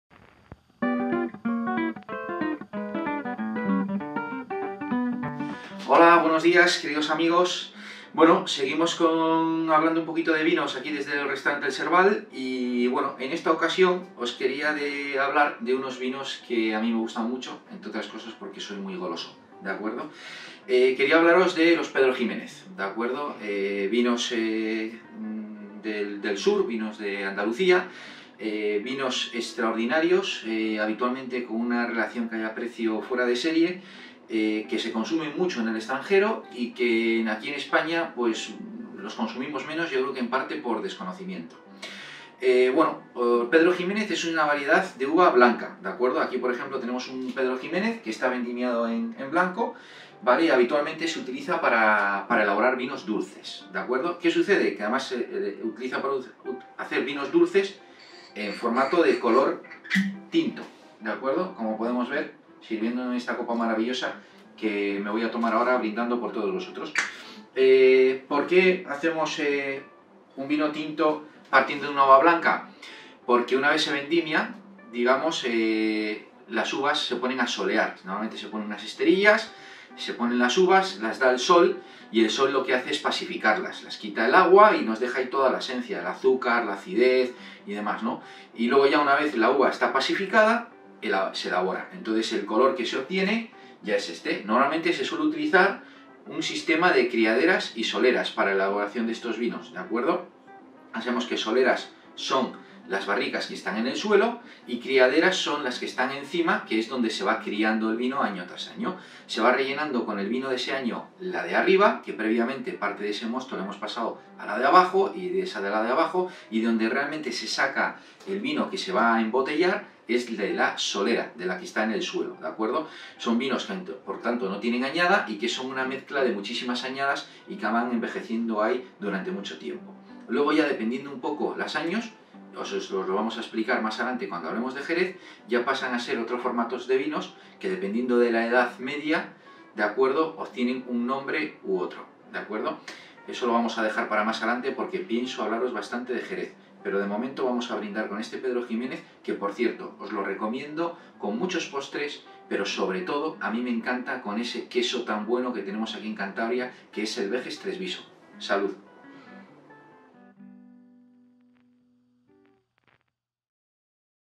Los Pedro Ximénez | El Serbal
El vino Pedro Ximénez se realiza con una variedad de uva blanca que produce un vino muy oscuro y se elabora secando las uvas al sol. ¿Quieres conocer más sobre este vino español? Toda la información nos la da el director del restaurante El Serbal, Rafael Prieto.

►🎥Grabación y Edición: Eva Vallejo